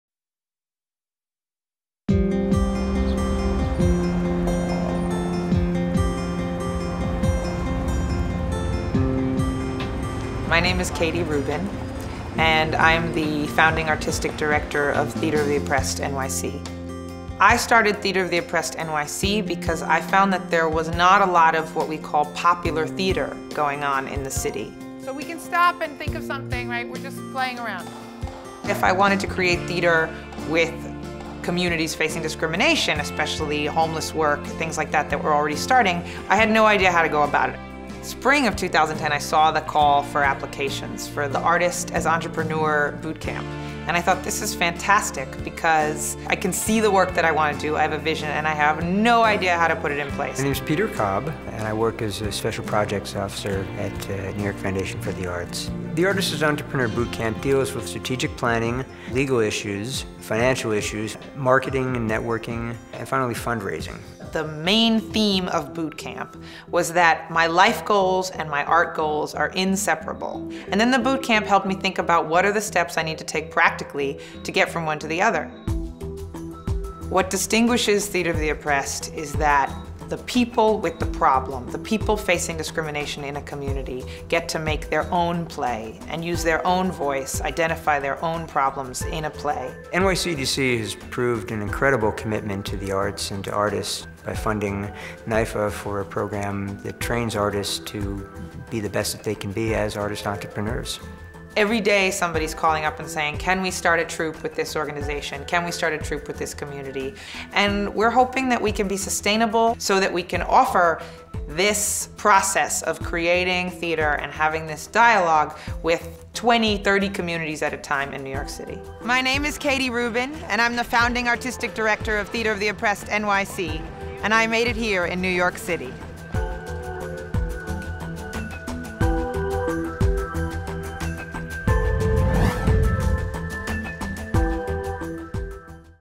NEW YORK CITY. MAKE IT HERE., featuring Theatre of the Oppressed NYC
Find out how Katy Rubin, Founder of Theatre of the Oppressed NYC, was able to marry her artistic side with her entrepreneurial side by enrolling in and graduating from the NYCEDC, NYFA Artist as Entrepreneur Boot Camp. It's a fascinating story of how, with training and mentoring, art and business can live hand-in-hand, and make it in New York City.